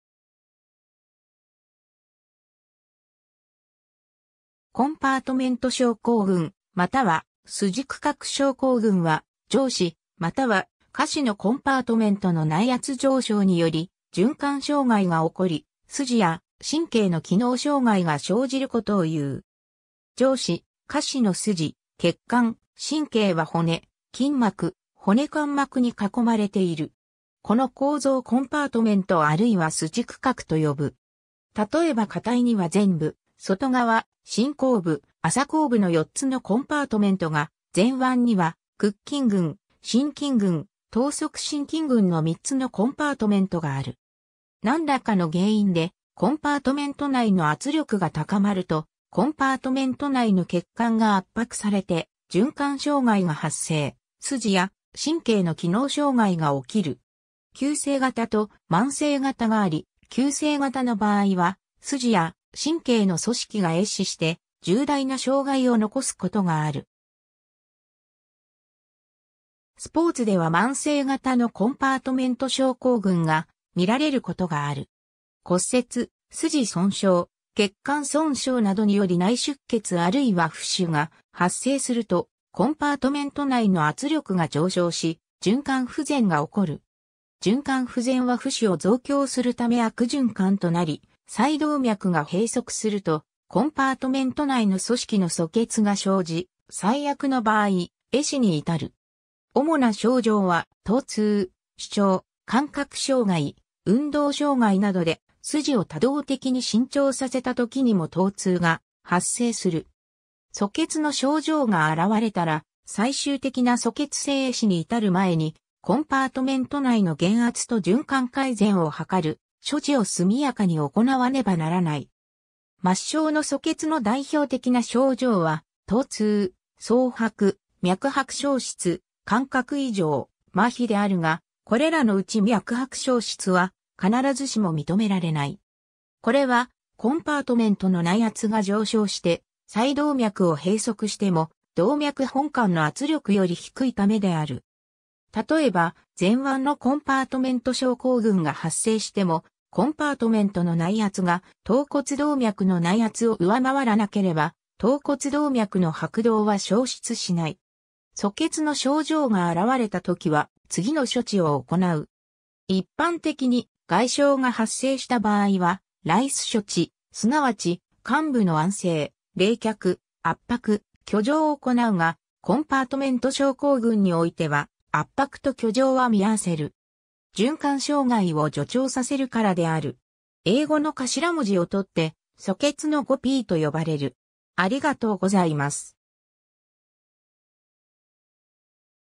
コンパートメント症候群
コンパートメント症候群（コンパートメントしょうこうぐん、compartment syndrome）、または筋区画症候群（きんくかくしょうこうぐん）は、上肢または下肢のコンパートメント（筋区画）の内圧上昇により循環障害がおこり、筋や神経の機能障害が生じることをいう。
 上肢、下肢の筋、血管、神経は骨、筋膜、骨間膜に囲まれている。
この構造をコンパートメント(compartment)あるいは筋区画と呼ぶ。
例えば下腿には前部、外側、深後部、浅後部の4つのコンパートメントが、前腕には屈筋群、伸筋群、橈側伸筋群の3つのコンパートメントがある。
何らかの原因でコンパートメント内の圧力が高まると、コンパートメント内の血管が圧迫されて循環障害が発生、筋や神経の機能障害がおきる。
 急性型と慢性型があり、急性型の場合は筋や神経の組織が壊死して重大な障害を残すことがある。
スポーツでは慢性型のコンパートメント症候群がみられることがある。
 骨折、筋損傷、血管損傷などにより内出血あるいは浮腫が発生すると、コンパートメント内の圧力が上昇し循環不全がおこる。
循環不全は浮腫を増強するため悪循環となり、細動脈が閉塞するとコンパートメント内の組織の阻血が生じ、最悪の場合、壊死に至る。
 主な症状は疼痛、腫脹、感覚障害、運動障害などで、筋を他動的に伸長させたときにも疼痛が発生する。
 阻血の症状が現れたら、最終的な阻血性壊死に至る前に、コンパートメント内の減圧と循環改善を図る処置を速やかに行わねばならない。
 末梢の阻血の代表的な症状は、疼痛(pain)、蒼白(paleness)、脈拍消失(pulselessness)、感覚異常(paresthesia)、麻痺(paralysis)であるが、これらのうち脈拍消失は必ずしも認められない。
これは、コンパートメントの内圧が上昇して細動脈を閉塞しても動脈本管の圧力より低いためである。
例えば、前腕のコンパートメント症候群が発生してもコンパートメントの内圧が橈骨動脈の内圧を上回らなければ、橈骨動脈の拍動は消失しない。
 阻血の症状が現れたときは、次の処置を行う。
 一般的に、外傷が発生した場合はRICE処置、すなわち患部の安静(rest)、冷却(icing)、圧迫(compression)、挙上(elevation)を行うが、コンパートメント症候群においては、圧迫と挙上は見合わせる。
循環障害を助長させるからである。
 ^ 英語の頭文字をとって阻血の5Pと呼ばれる。